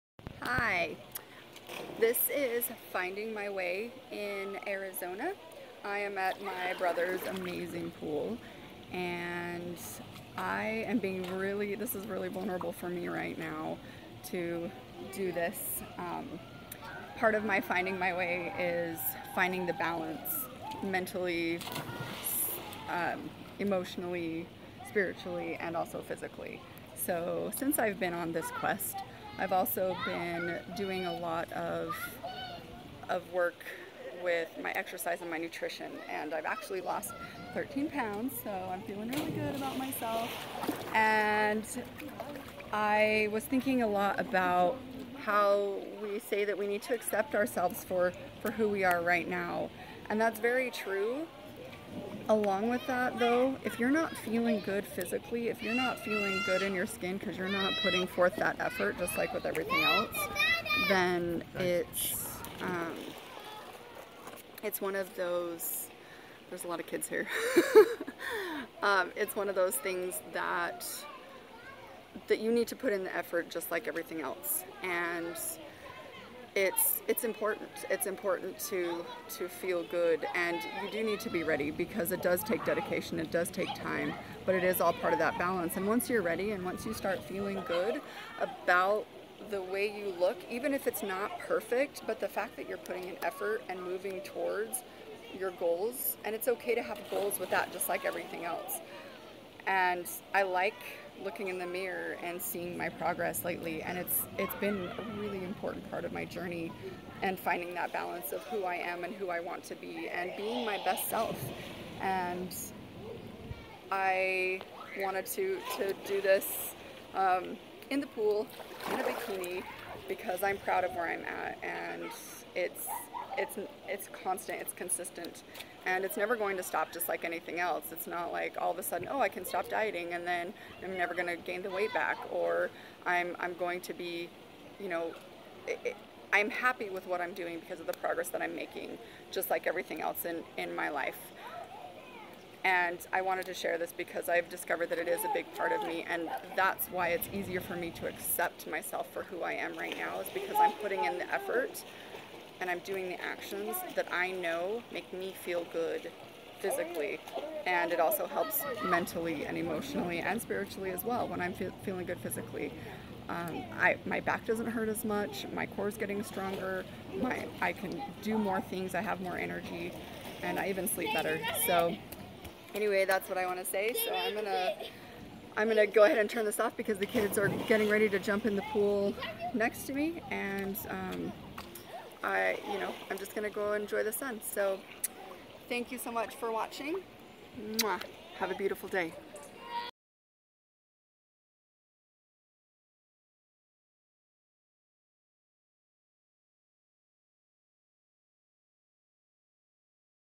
Finding My Way in Mesa, Arizona
Feeling good physically is just as important to find balance in my life as everything else. I've been working hard and am being vunerable to get out of my comfort zone and grow!